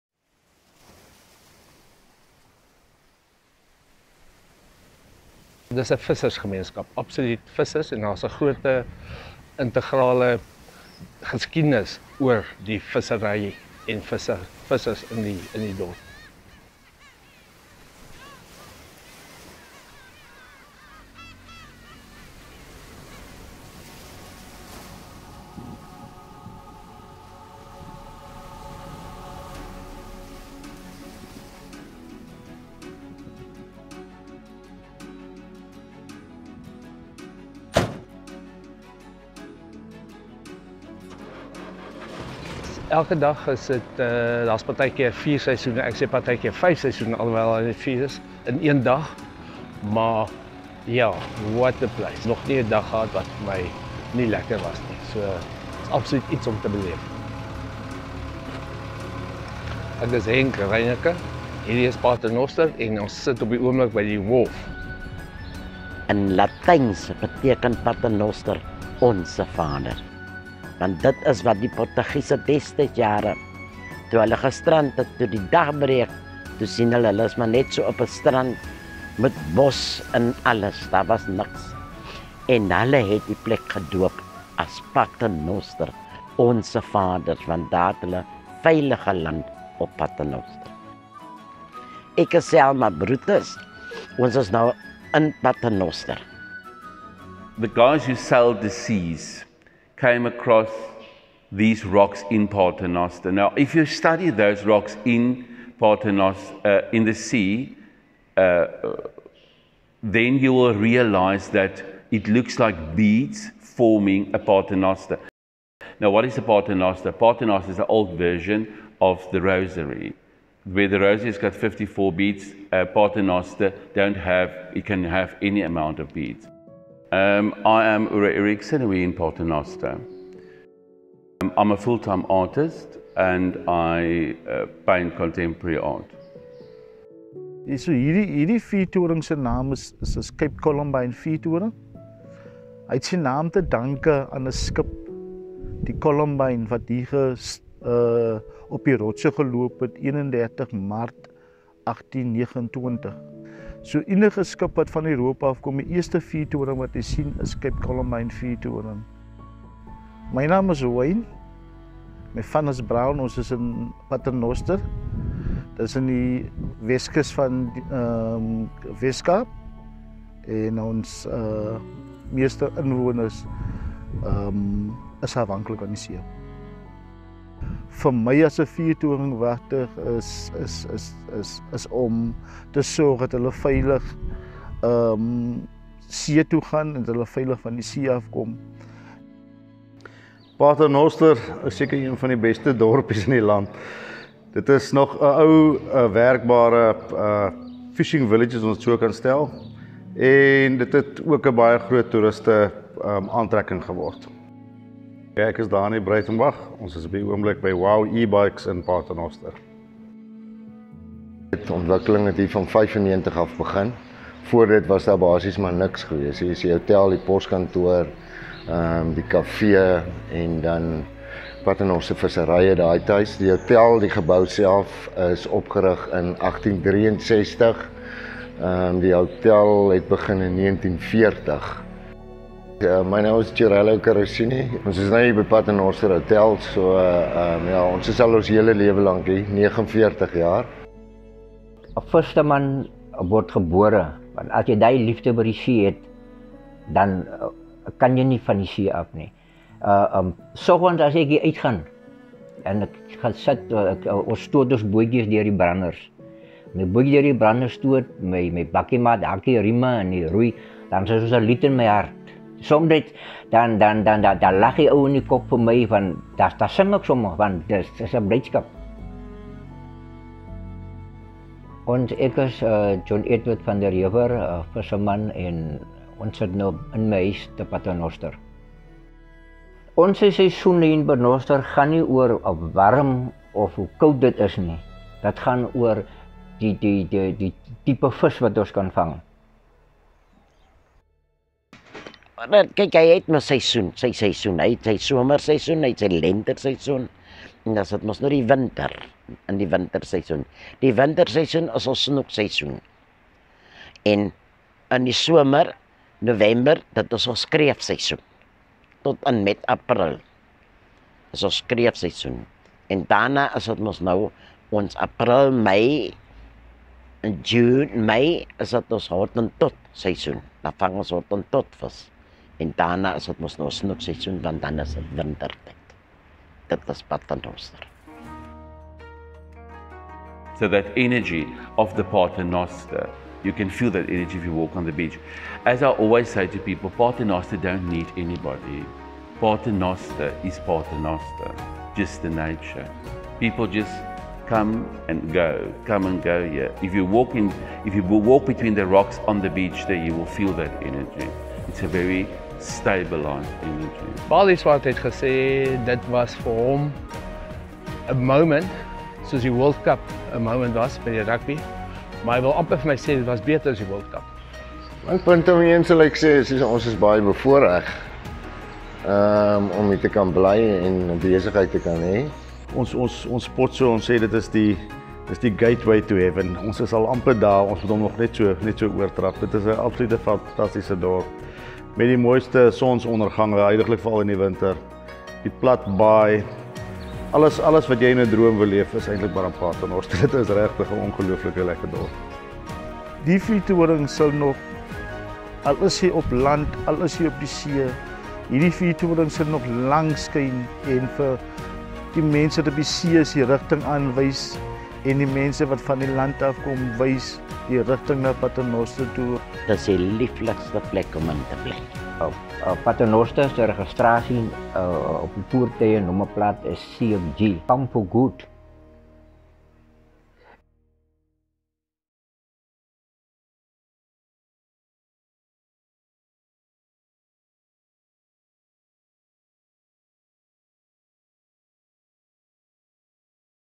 Paternoster, Western Cape
Paternoster is one of the oldest fishing villages on the West Coast of South Africa. It is situated 15 km north-west of Vredenburg and 145 km north of Cape Town, at Cape Columbine between Saldanha Bay and St Helena Bay. The town covers an area of 194.8 hectare and has approximately 1883 inhabitants.
The origin of the name remains unknown. Many people believe that the name, which means ‘Our Father’ in Latin, refers to prayers said by Catholic Portuguese seamen when they became shipwrecked.It appears as St. Martins Paternoster on an old map of Pieter Mortier so the name may be derived from Paternoster Row in the City of London which is adjacent to St. Martins Court.Other people believe it refers to the beads that the Khoi tribe wore that were called Paternosters.

0:00 Weskus Tourism or
0:22 Stofpad Tours
1:38 Uro Erichsen Artist
2:19 Cape Columbine Lighthouse
3:13 WOW Ebikes
3:34 Paternoster Hotel